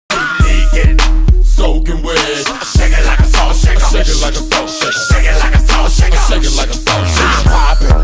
Dredge Fishing for Sailfish using Marea Dredges
Here's a short clip of one of the top tournament crews deploying our realistic baitfish dredges during a local tournament.